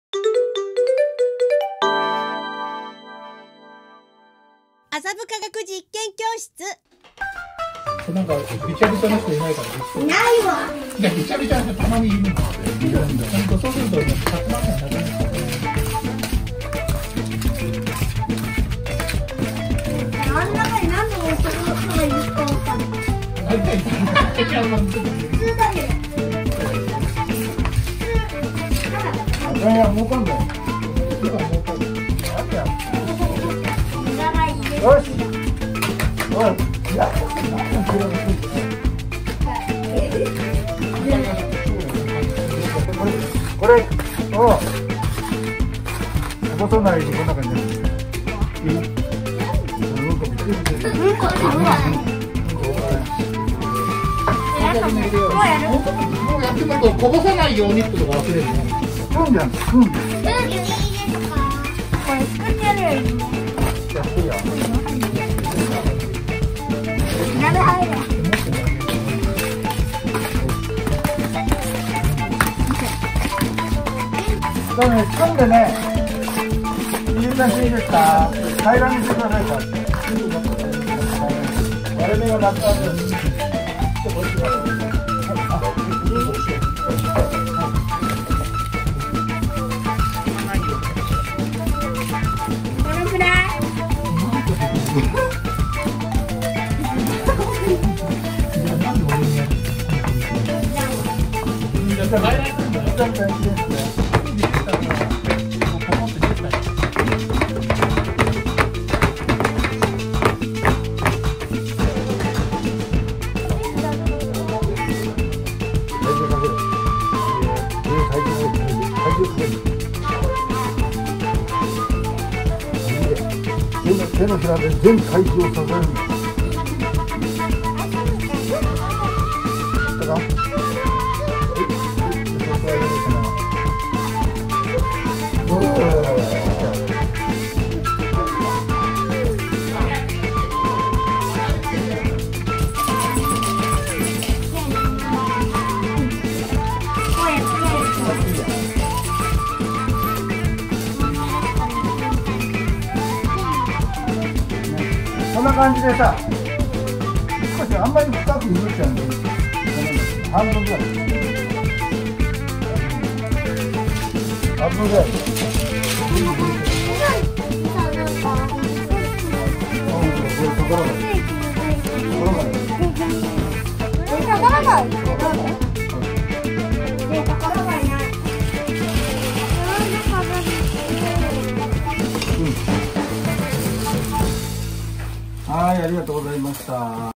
コンクリートの手形づくり：一年生：理科実験 【麻布科学実験教室】
セメントとコンクリートの違い、知ってますか？(^^;
そんな素朴な疑問もこうやって、砂遊びをしながら体験学習。
砂遊び大好き一年生が一生懸命スプーンで砂を混ぜます。
だんだん硬くなる。ペチャペチャすると綺麗になる。
どんどんすると泡が出てくる。　不思議な現象が次々に。。
そして、手を押し付けて型を取って、飾り付けをして出来上がり。
１週間教室で乾燥させて、次の週に渡します。
　１年生の小さな手の時のメモリー

【麻布科学実験教室】@jikken_gram